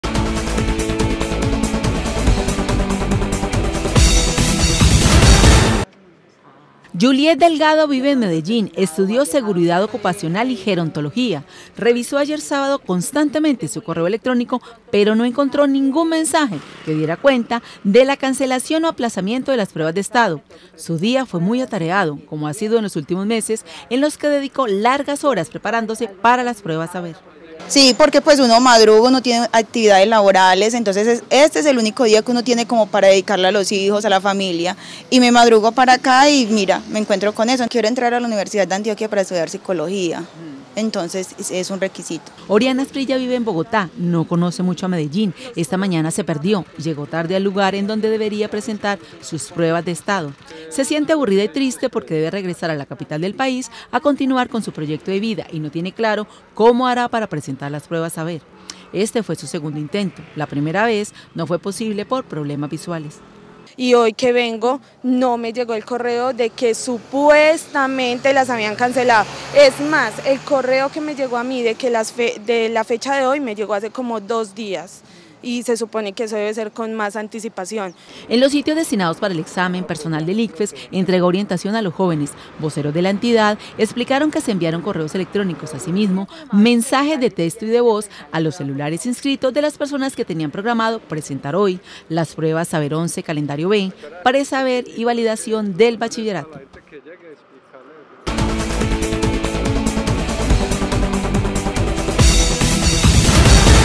Despistados madrugaron a las pruebas Icfes - Teleantioquia Noticias
Aunque el presidente de la República, Iván Duque, informó, a través de redes sociales, y de los medios de comunicación, que las pruebas Saber  programadas para hoy se aplazaban para evitar la propagación del coronavirus, algunas personas no se enteraron y madrugaron a cumplir con la cita.